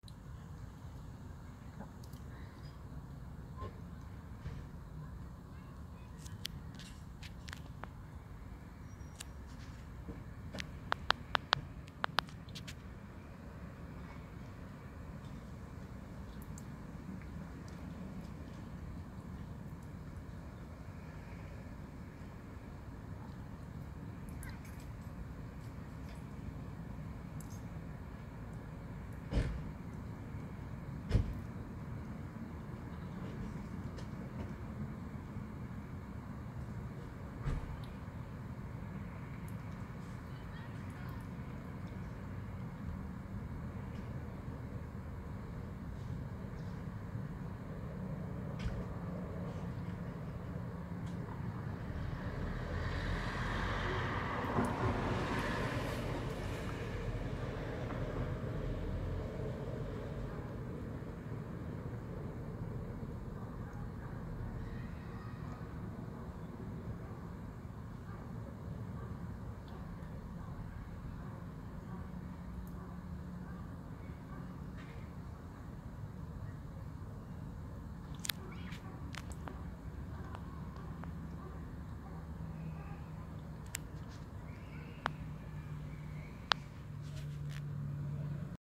A Spider On A Web Outside My Window.
I Spotted This Spider On A Web Covering The Window Next To The Front Door.